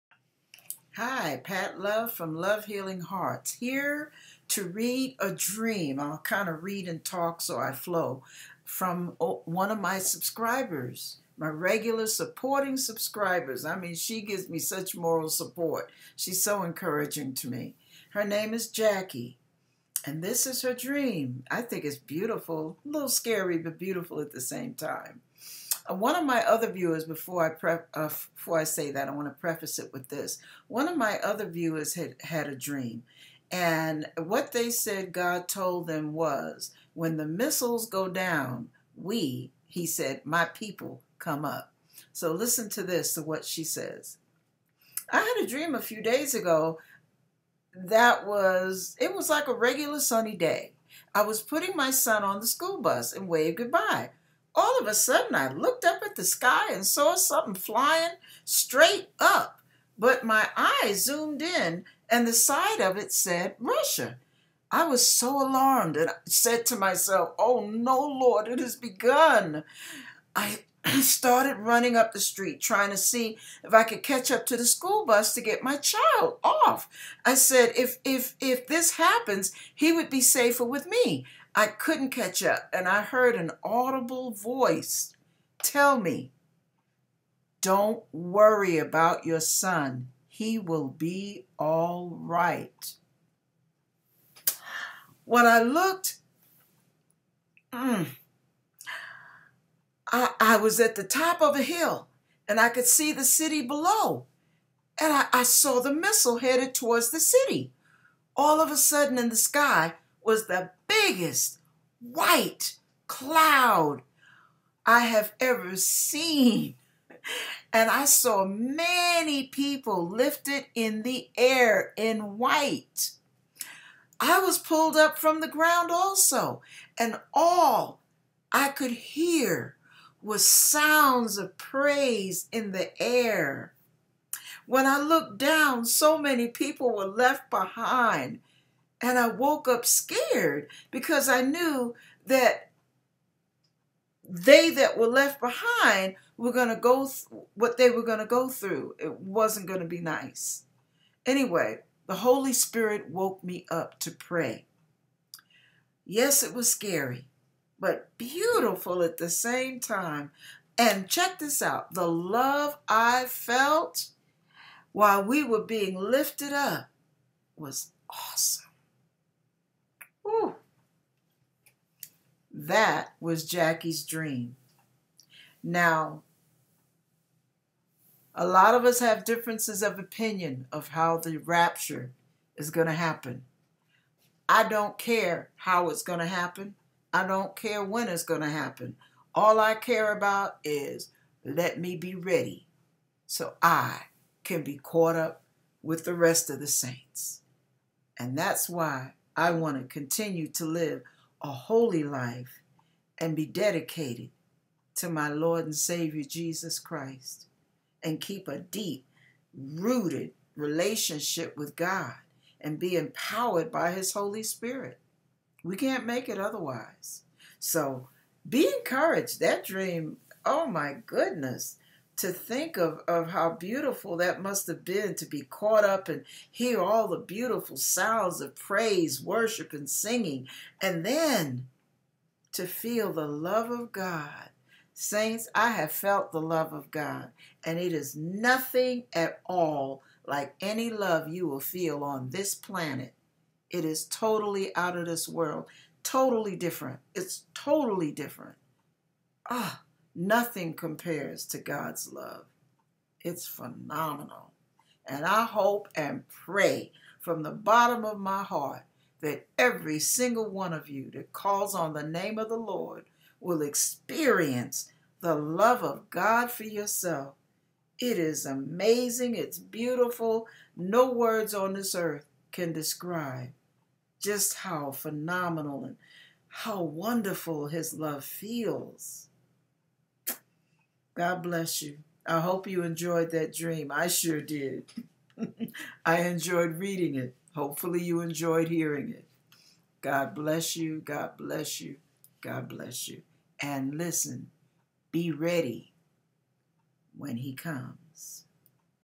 END-TIME Dream: RUSSIAN MISSILES! * RAPTURE! * "When MISSILES Go Down MY People Go Up!"
End Time Dream . Missile, Russia! Rapture, Amazing eerie Sounds! (LHH) Pat Love tells a dream from Jackie, a Viewer / Subscriber. * GodsIn2Love (tm) * ..V..V..More VIDEO LINKS & * BOOKS * Below...V..V...

1.* * ."THAT STILL BLOWS MY MIND"
2.* * ."AN EPIC JOURNEY INTO THE SUPERNATURAL"

- GOD;S ONLINE Church of Love GodsIn2love because God is love. GodSense + pats2cents = greatSense TalkVideo